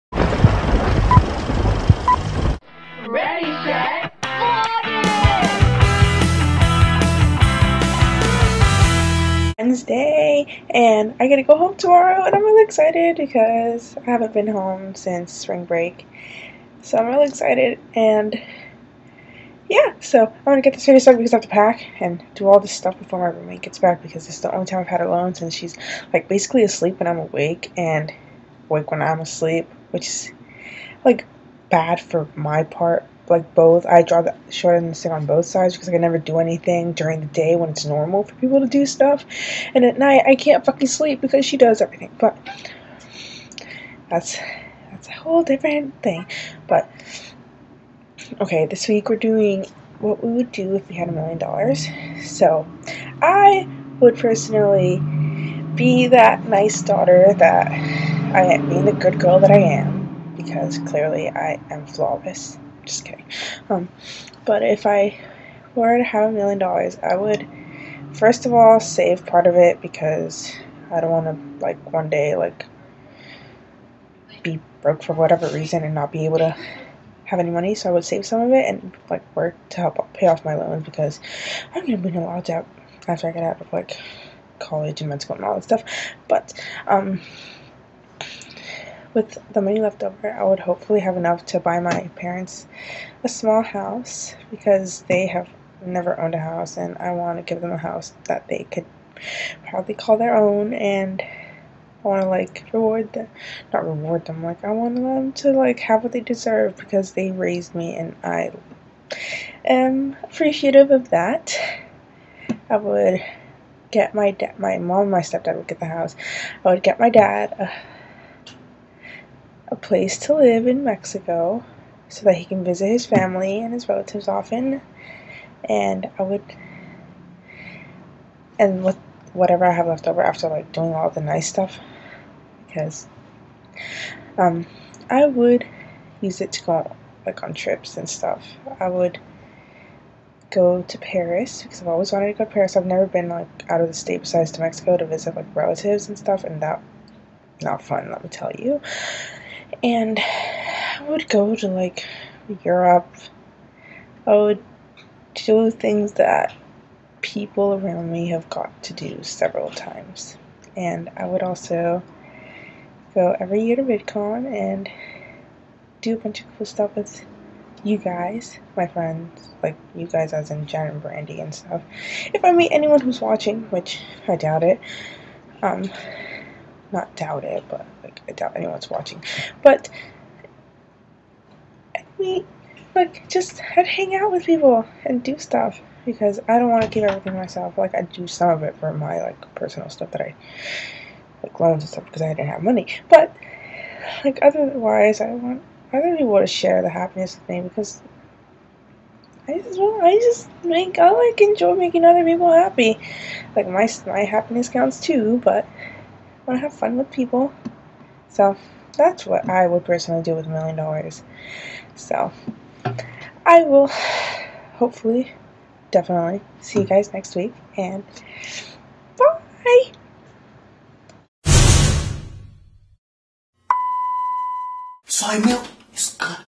If I Had a Million Dollars/ Won the Lottery
I'm a nice person, I swear!! I just really like that video!!! :)
P.S.S. I read/heard the topic wrong, I guess, but I would do the exact same things, so yeah.